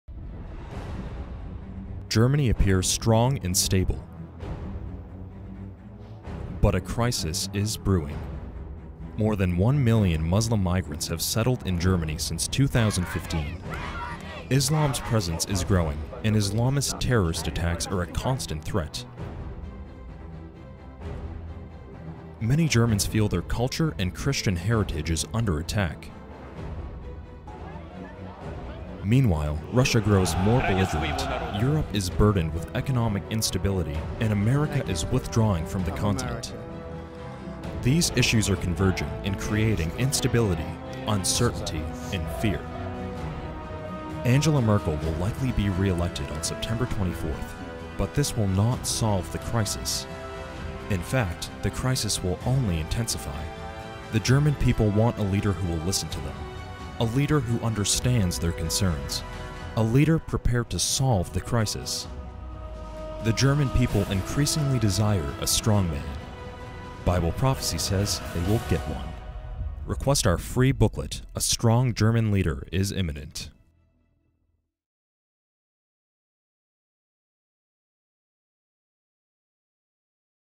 Germany: A Strong Leader Is Imminent
Germany appears strong and stable, but a crisis is brewing. More than 1 million Muslim migrants have settled in Germany since 2015. Islam’s presence is growing and Islamist terrorist attacks are a constant threat. Many Germans believe their culture and Christian heritage are under attack.

Meanwhile, Russia grows more belligerent, Europe is burdened with economic instability, and America is withdrawing from the Continent.

These issues are converging and creating instability, uncertainty and fear.

Angela Merkel will likely be reelected on September 24, but this will not solve the crisis. In fact, the crisis will only intensify!

The German people want a leader who will listen to them, a leader who understands their concerns, a leader prepared to solve the crisis. The German people increasingly desire a strongman.

Bible prophecy says they will get one.

Request our free booklet A Strong German Leader Is Imminent to learn more about what Bible prophecy has to say Germany.